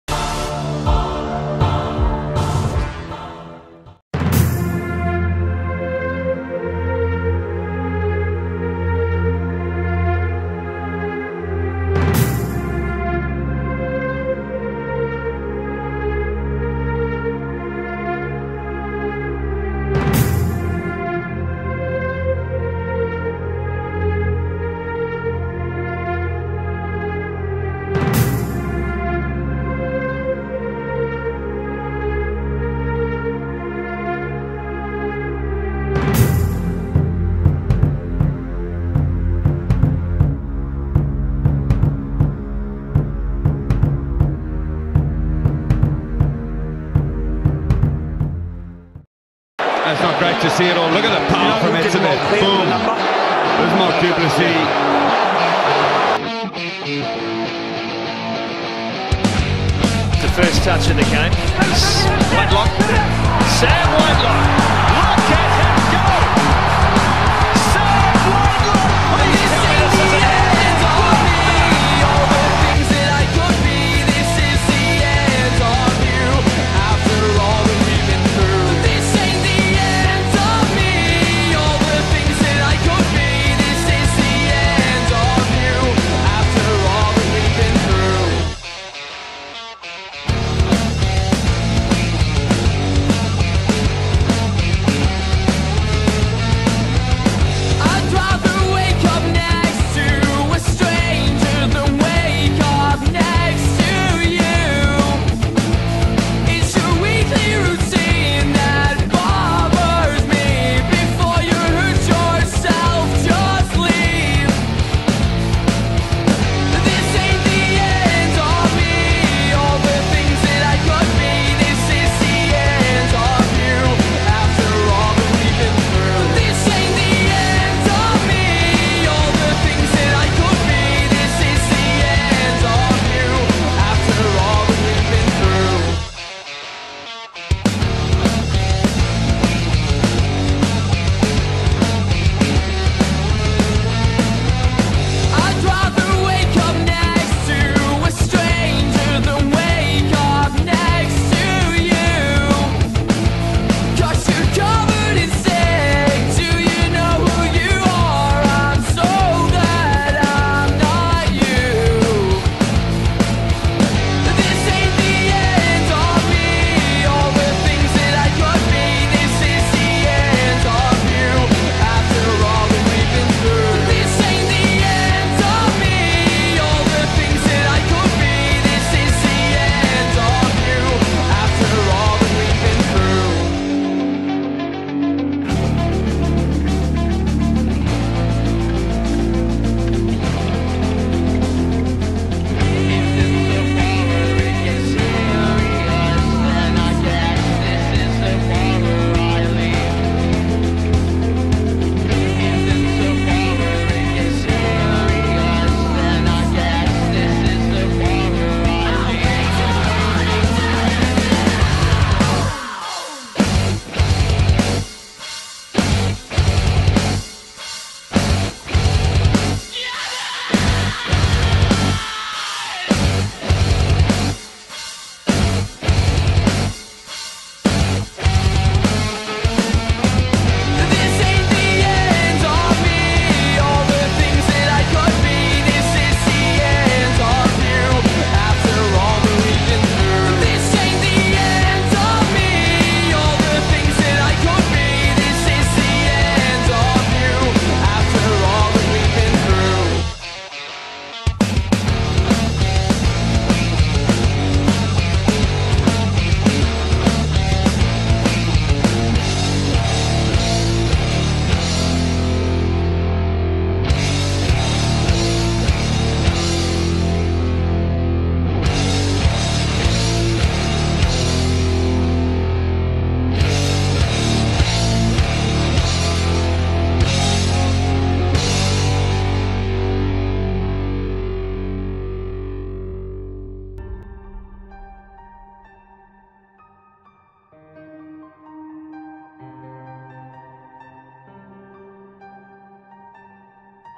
Rugby Tribute : Locks/Second Row " Giants among humans" Rugby Highlights " Big hits e Runs"
The locks, or second row , the tallest players on the field, you're ever trying to avoid a collision with them, because you know that they are going to smash you! In this video you can see the biggest hits and the most powerful runs of those giants!

I make no profit from these montages, I make them for fun and only fun.